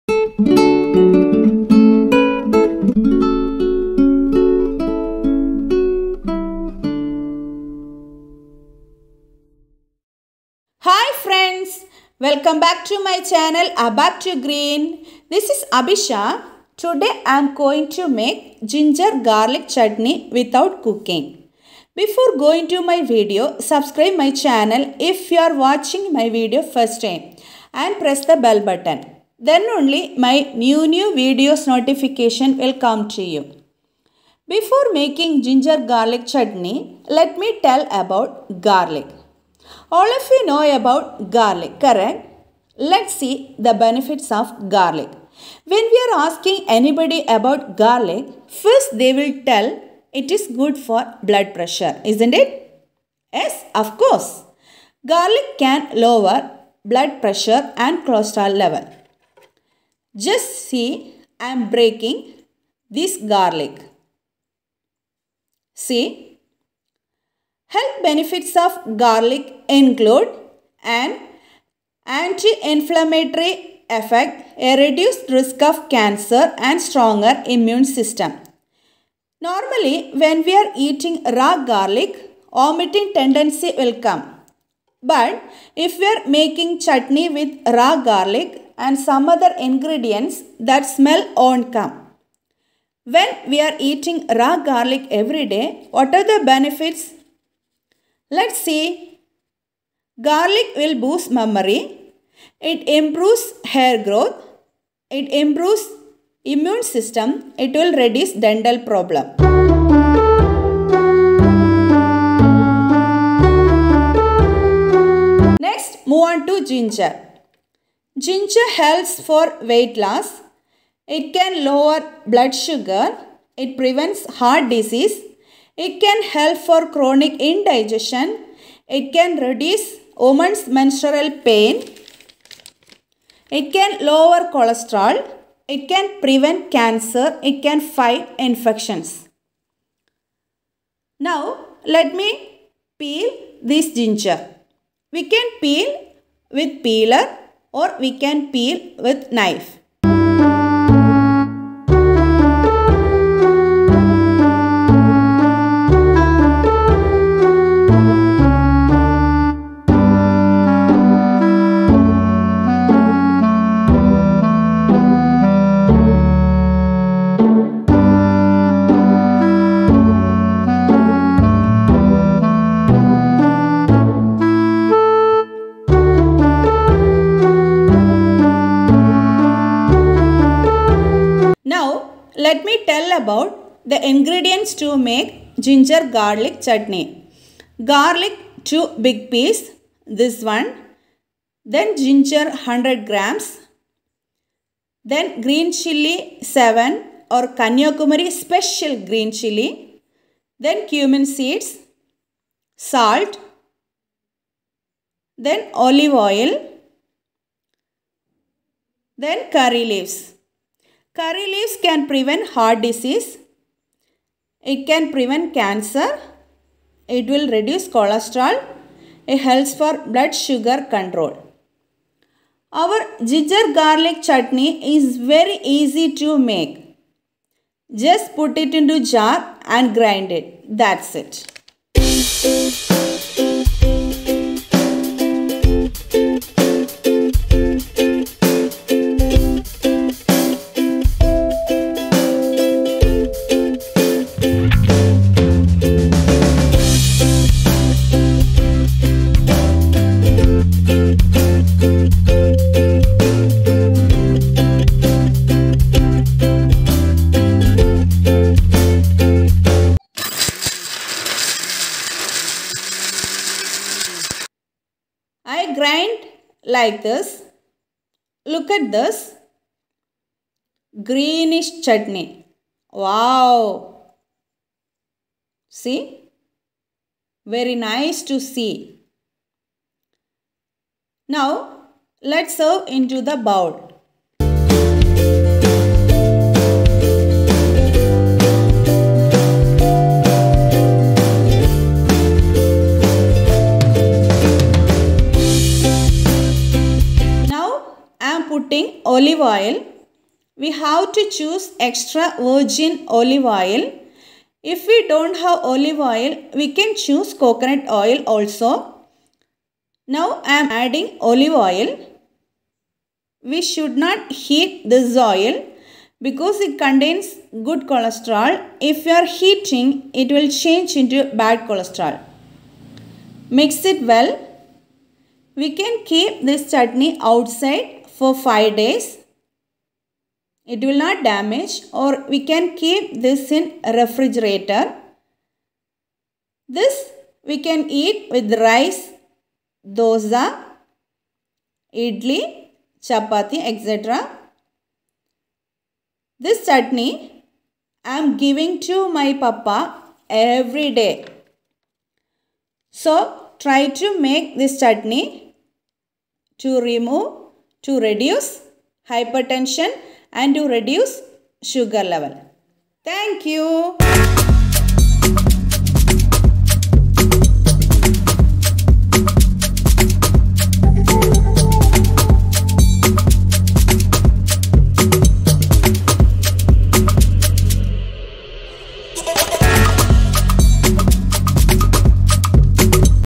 How to make FRESH GINGER GARLIC CHUTNEY without cooking.
Hi friends.
This is ABACTIVE GREEN here to show you how to prepare FRESH GINGER GARLIC CHUTNEY just without  cooking.
ITS a healthy dish to reduce hypertension and sugar level.
Its mainly good for aged people.
INGREDIENTS
                                                *ginger( 100 g)
                                                *garlic(2 big piece)
                                                *olive oil
                                                *salt
                                                *cumin seeds
                                                *green chilli ( 7 pieces)
So enjoy the video by watching.